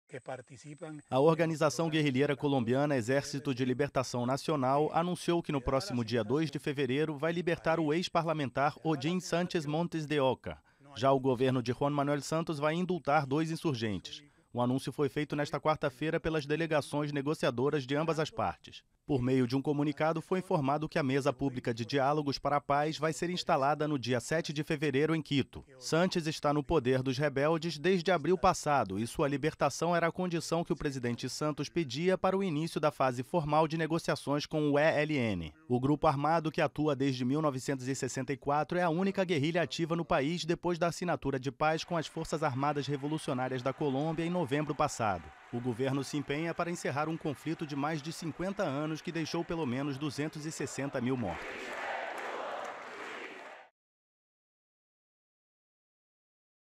ELN anuncia libertação de parlamentar sequestrado
A organização guerrilheira colombiana Exército de Libertação Nacional anunciou que vai libertar o ex-parlamentar Odín Sánchez Montes de Oca que é refém desde abril passado. Sánchez está em poder dos rebeldes desde abril passado e sua libertação era a condição que o presidente Juan Manuel Santos pedia para o início das negociações.